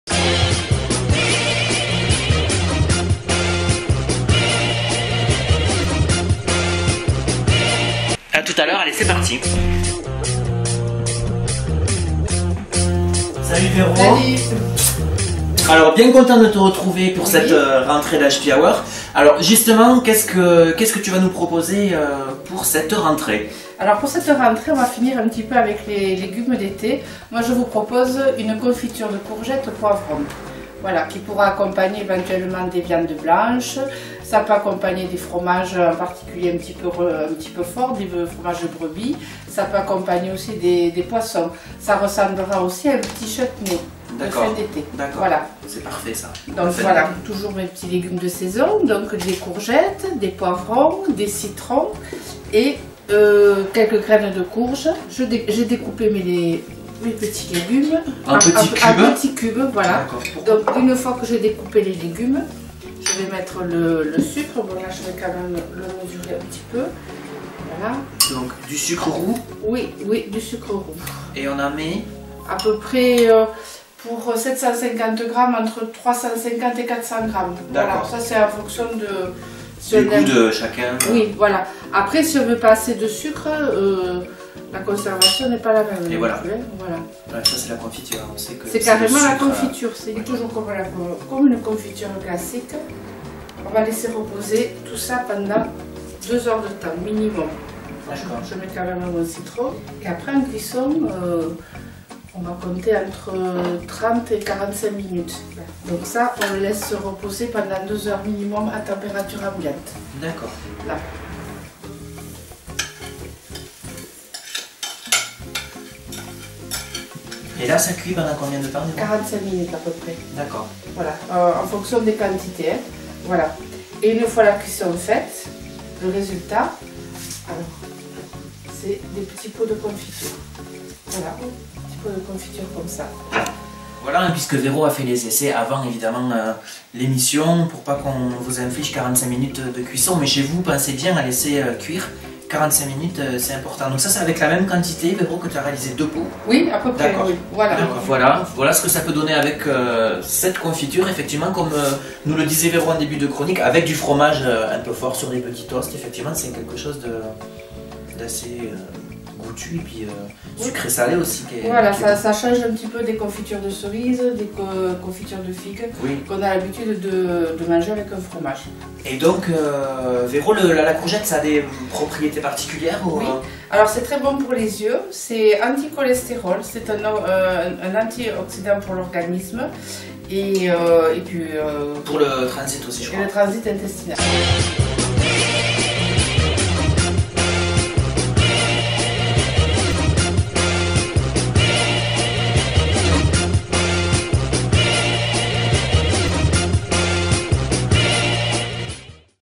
La Cuisine de Véro #01 Confiture Courgettes Poivrons | HPyTv La Télé des Pyrénées
Dans HPy Hour, le mag hebdo de HPyTv, on mange bon et sain grâce à Véronique Pujalte. Avec les recettes de Pom Cuisine, le bio se conjugue avec le bon et les mélanges sont originaux.
La preuve avec la recette de confitures de courgettes et poivrons que Véro vous détaille en compagnie de Steeve.
Production : pyreneesinfo.fr - HPyTv La Télé des Pyrénées